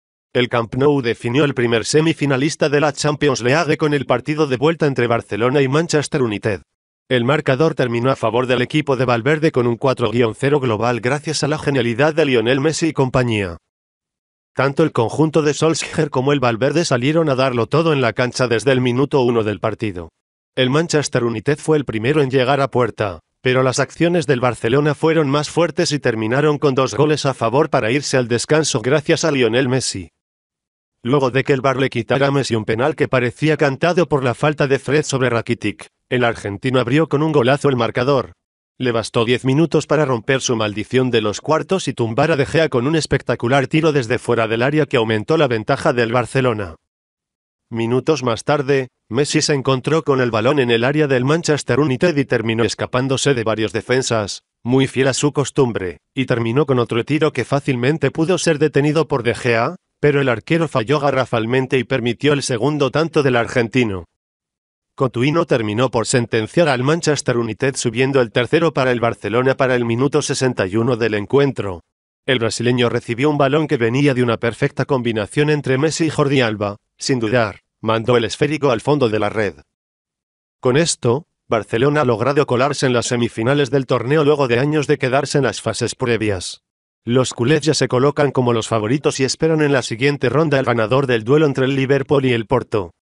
Con un imparable Messi Barcelona vence 3-0 al Manchester United y lo elimina de la Champions League
El Camp Nou definió el primer semi finalista de la Champions League con el partido de vuelta entre Barcelona y Manchester United. El marcador terminó a favor del equipo de Valverde con un 4-0 global gracias a la genialidad de Lionel Messi y compañía.

Tanto el conjunto de Solskjaer como el Valverde salieron a darlo todo en la cancha desde el minuto uno del partido. El Manchester United fue el primero en llegar a puerta, pero las acciones del Barcelona fueron más fuertes y terminaron con dos goles a favor para irse al descanso gracias a Lionel Messi.

Luego de que el VAR le quitara a Messi un penal que parecía cantado por la falta de Fred sobre Rakitic, el argentino abrió con un golazo el marcador. Le bastó 10 minutos para romper su maldición de los cuartos y tumbar a De Gea con un espectacular tiro desde fuera del área que aumentó la ventaja del Barcelona.

Minutos más tarde, Messi se encontró con el balón en el área del Manchester United y terminó escapándose de varios defensas, muy fiel a su costumbre, y terminó con otro tiro que fácilmente pudo ser detenido por De Gea, pero el arquero falló garrafalmente y permitió el segundo tanto del argentino.

Cotuinho terminó por sentenciar al Manchester United subiendo el tercero para el Barcelona para el minuto 61 del encuentro. El brasileño recibió un balón que venía de una perfecta combinación entre Messi y Jordi Alba, sin dudar, mandó el esférico al fondo de la red.

Con esto, Barcelona ha logrado colarse en las semifinales del torneo luego de años de quedarse en las fases previas. Los culés ya se colocan como los favoritos y esperan en la siguiente ronda al ganador del duelo entre el Liverpool y el Porto.